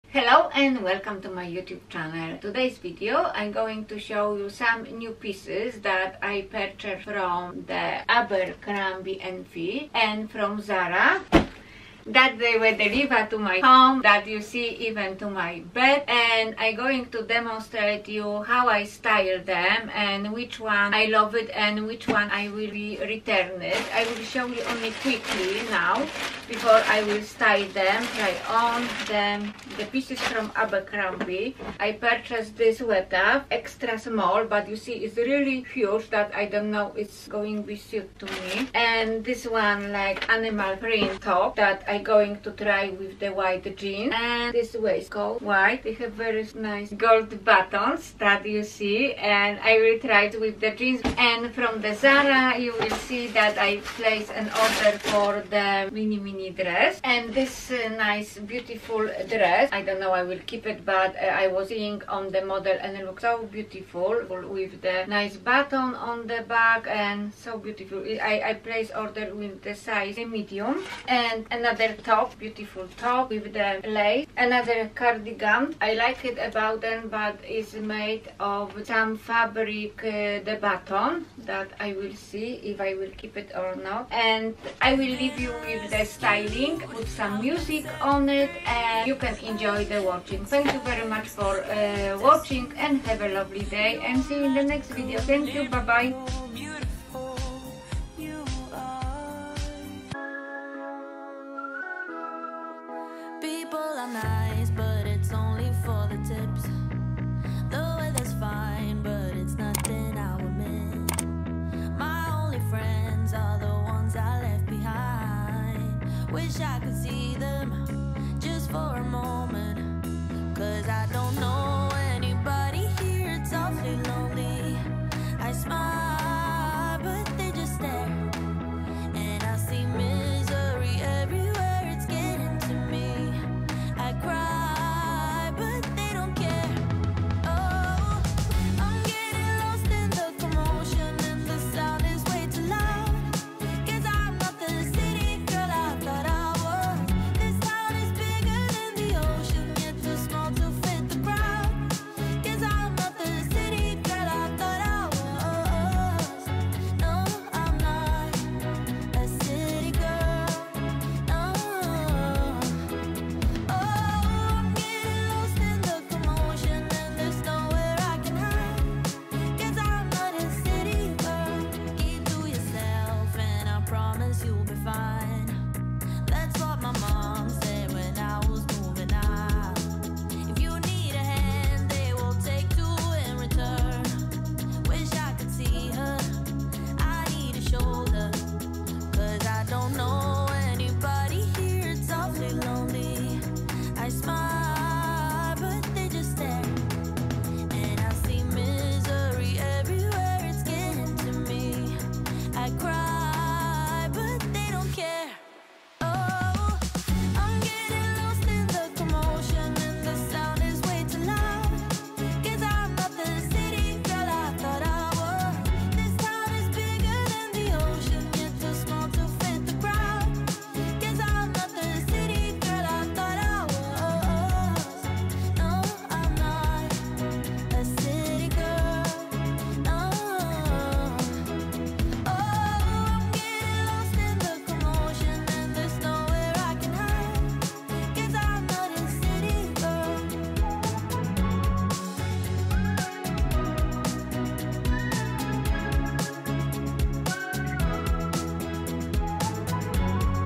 Zara and Abercrombie and Fitch What New TRY ON HAUL.
Hello everyone!
I have ordered a few new pieces from both brands because I have had big hope that will add up a bit of trendy vibe to my wardrobe.
But I can only say that this time it was not the best pick for me. My Imagination and reality did not match this time.

I have put ref number of the clothes and size which I have been tryed.
Annaxx

House Of Cb And Zara Holiday Outfits Try-on Haul"